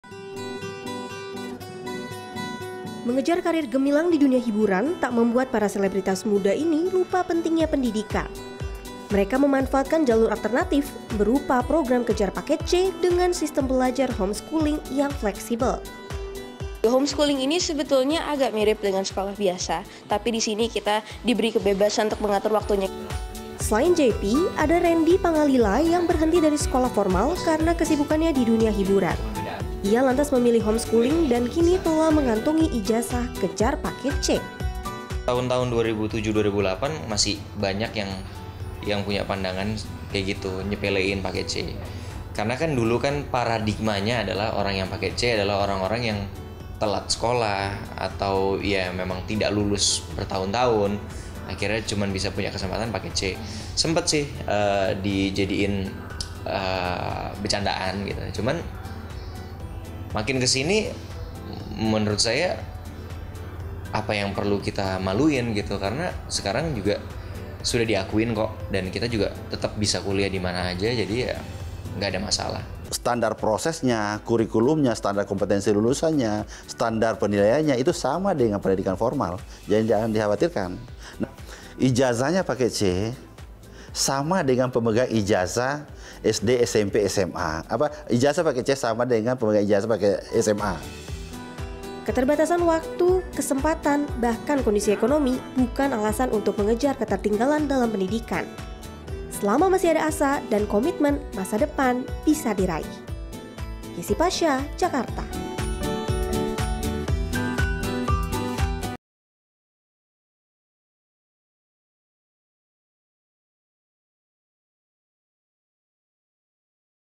Kejar Ijazah Dengan Paket C
Pemain film dan para pesohor yang tidak sempat bersekolah formal, menempuh jalur  "Alternatif" untuk menyelesaikan pendidikan. Mereka mengikuti ujian kejar paket, agar bisa mengantongi ijazah dan meneruskan pendidikan ke jenjang yang lebih tinggi.

Like & Follow Facebook:
CNN Indonesia

cnnindonesia